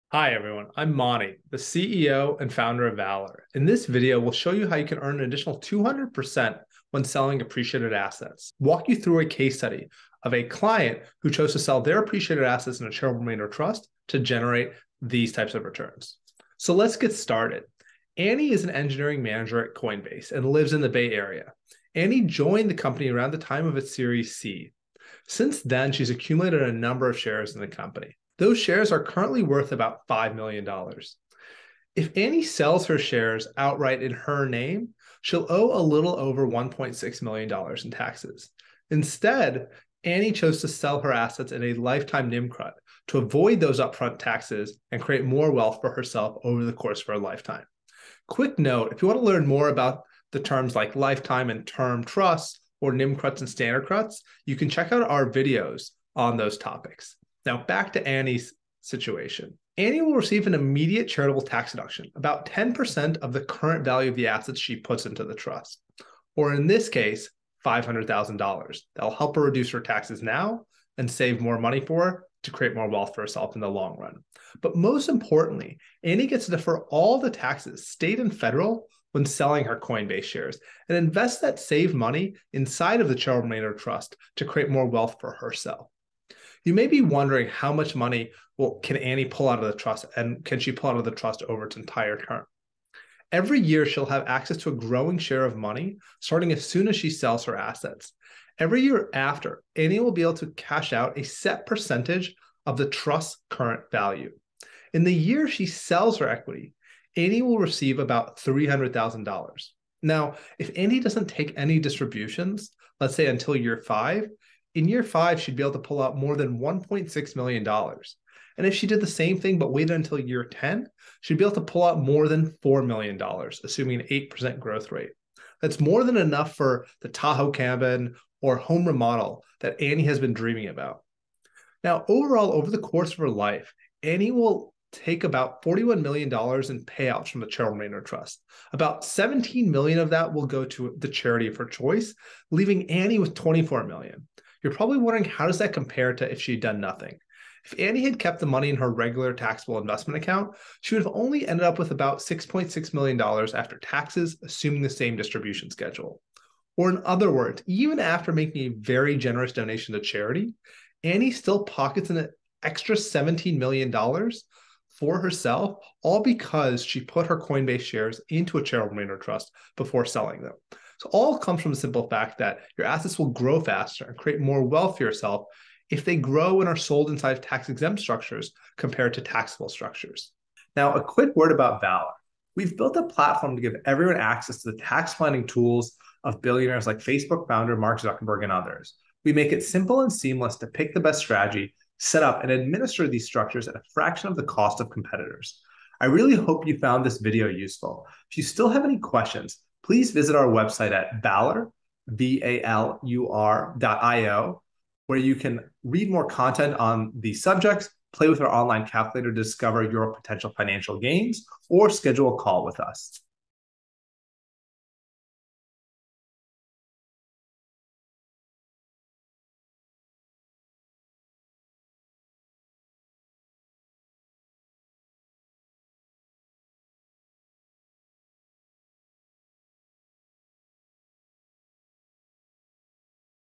How to Pocket EXTRA MONEY When Selling Appreciated Assets? A CRT Might Help!
In this video, we'll analyze a real-life example in-depth to help you understand how to win extra money when selling appreciated assets.
We'll explain how you can use a Charitable Remainder Trust (or CRT) to earn an additional 200% when selling your appreciated assets.

00:00 - Introduction
00:18 -  Analyzing a Coinbase example
00:48 - Learn more section
00:57 - How would a Charitable Remainder Trust impact Coinbase?
02:23 -What if Coinbase didn't set up Charitable Remainder Trust?
03:00 - Who we are

If you want to learn more, you can check out our videos or:
-Read through our content: